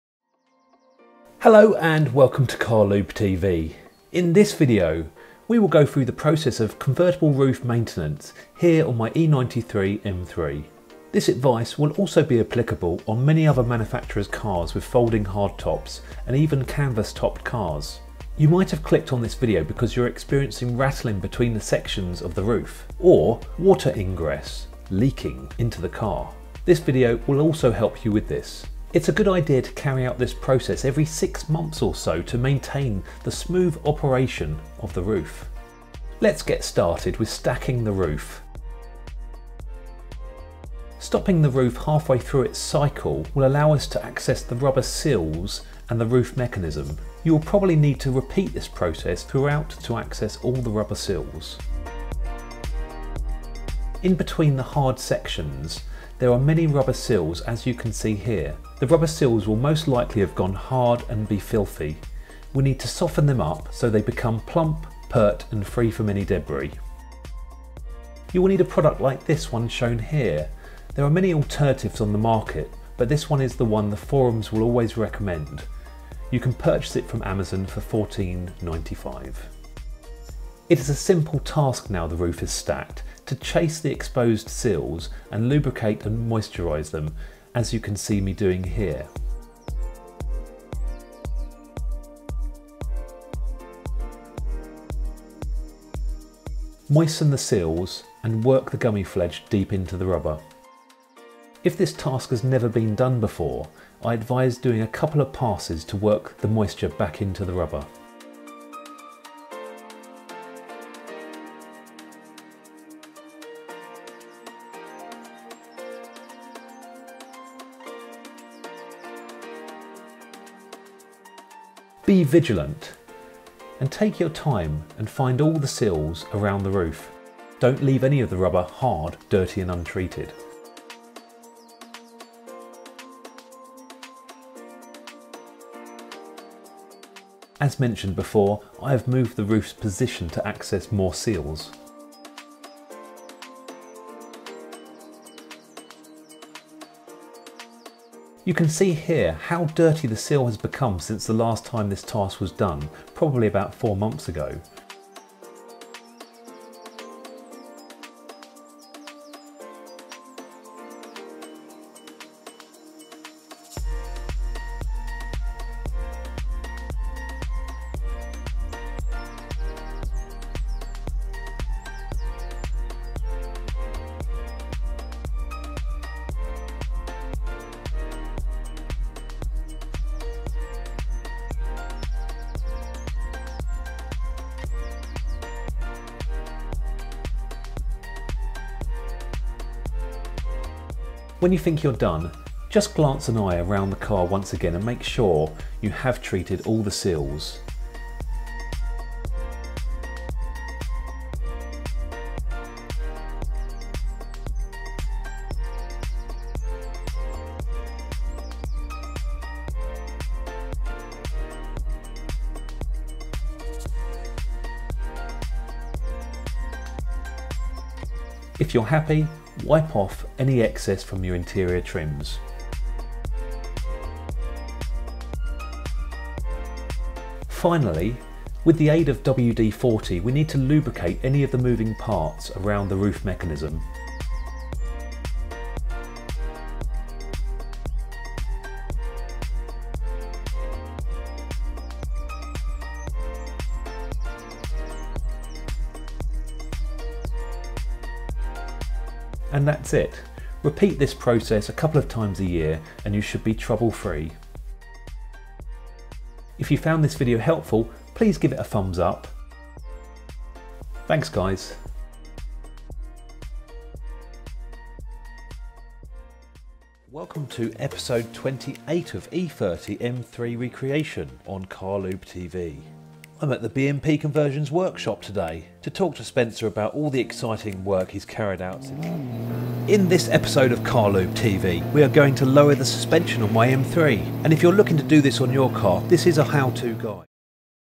How to look after a convertible roof - Lubricate rubber seals with Gummi Pflege - BMW - Fix creaking
In this video I will show to maintain the roof on your convertible car, it’s important to keep the rubber roof seals lubricated to avoid creaking, vibration, leaks etc, in this how I will show you how.

Please feel free to follow CarlubeTV on other social media networks.